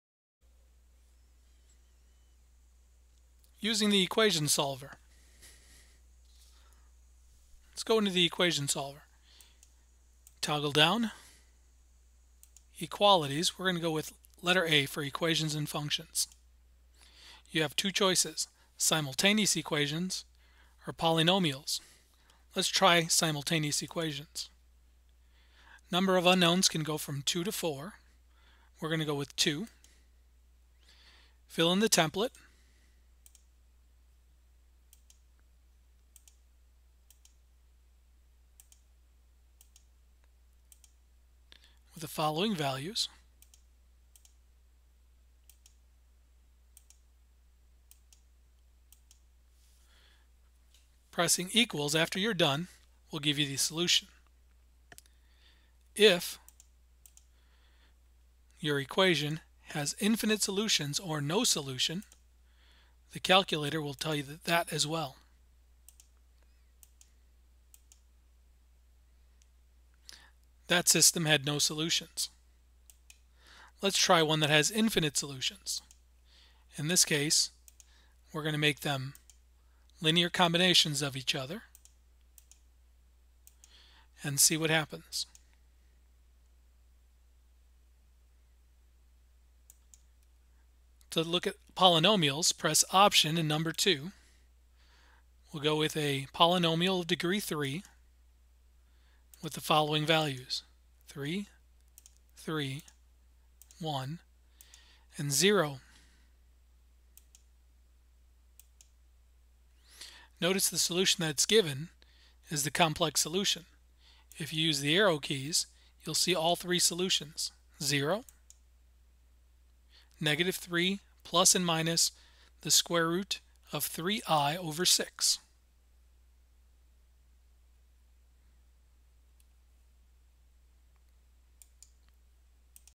ClassWiz Calculator Tutorial - Algebra 4-7 Equation solver
This video shows basic operation of CASIO ClassWiz for Equation solver.

[Compatible models ] fx-570EX, fx-991EX
[Related Topics] Algebra, Equation solver